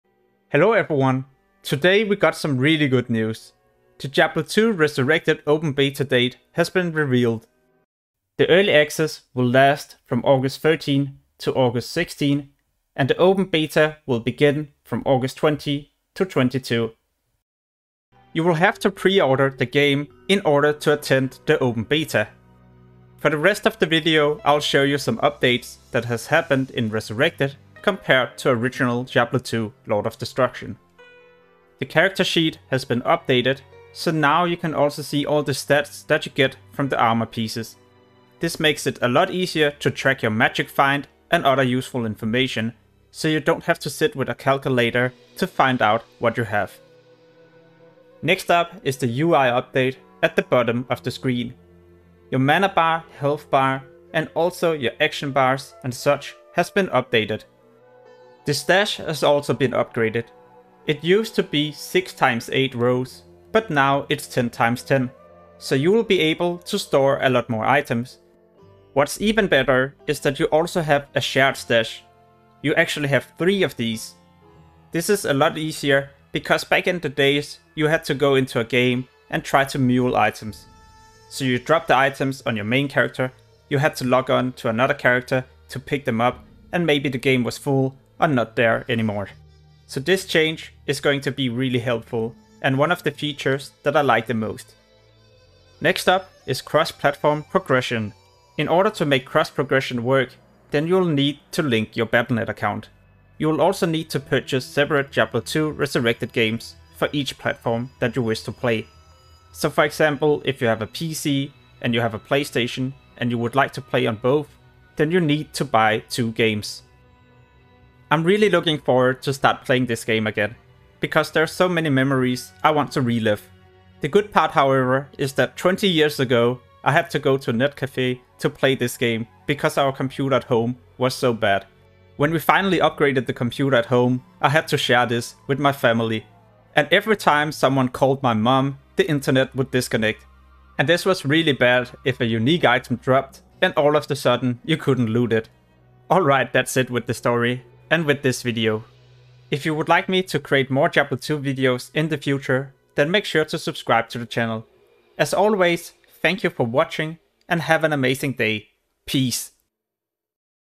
Diablo 2 Resurrected Beta and Release Date
The diablo 2 resurrected beta and launch date has official been confirmed by blizzard.